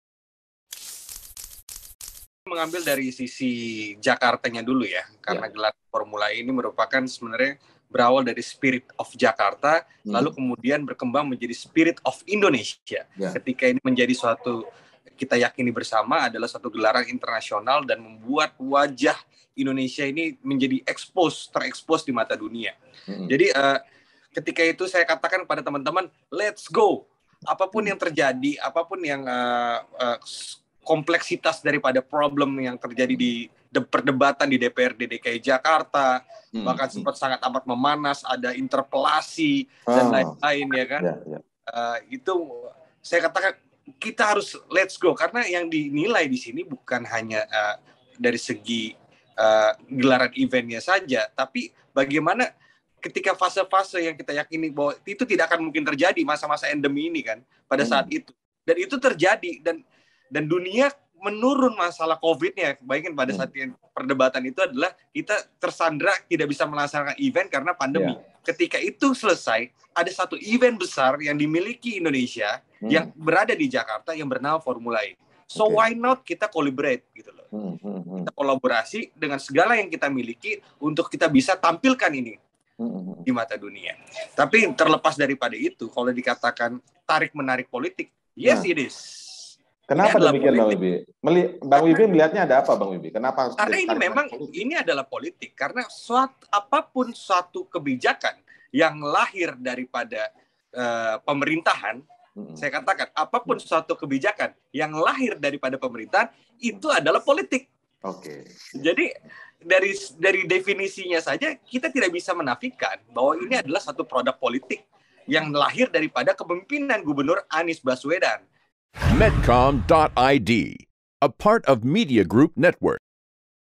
Berkat Formula E, Wajah Indonesia Terekspos Dunia
Gelaran Formula E berhasil dilaksanakan. Ajang balap mobil listrik dunia itu memberikan dampak besar bagi Indonesia. Ketua Fraksi NasDem DPRD Jakarta Wibi Andrino mengatakan bahwa event internasional ini membuat wajah Indonesia terekspos ke dunia. Wibi mengakui tak mudah menyelenggarakan Formula E karena banyak tantangan termasuk dihadang DPRD Jakarta sebagai mitra pemda, juga sejumlah fraksi yang mengajukan hak interpelasi. Menurutnya, wajar jika tarik menarik politik terjadi pada penyelenggaraan Formula E. Pasalnya, event tersebut merupakan program yang digagas oleh Pemda DKI Jakarta.